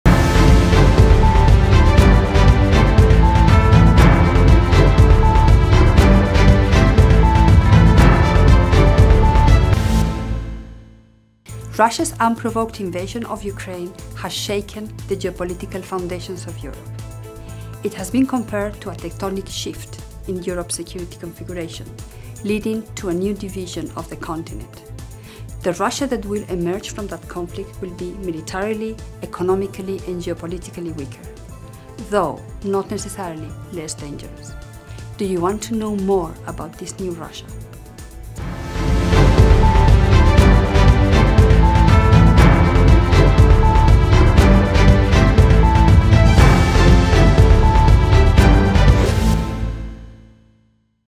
Russia, quo vadis? (Ten issues to watch in 2023)
➡️ Ten issues to watch in 2023: the 7th edition of our annual in-depth analysis is online!

EPRS identifies and frames some of the top key issues that might feature prominently in public debate and on the political agenda of the European Union this year.

In times of crises and war, what are the challenges we must face and choices we have to make? To know more, dig deeper into the ten issues to watch in 2023.